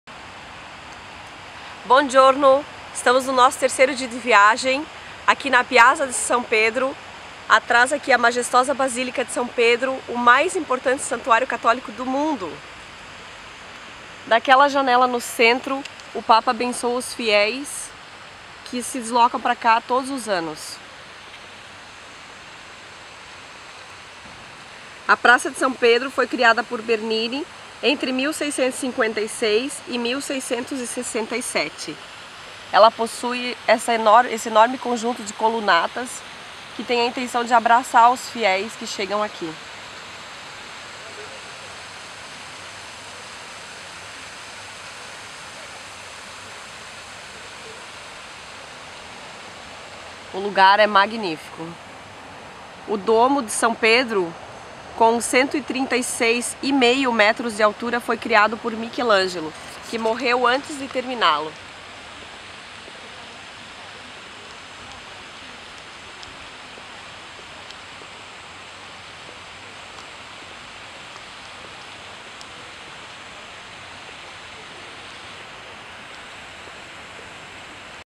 BASÍLICA DE SÃO PEDRO, Vaticano (Itália) [HD]
Vídeo gravado em 2012, na Basílica de São Pedro, em Vaticano (Itália).

"Video recorded in 2012 at St. Peter Basilic in Vatican (Italy)".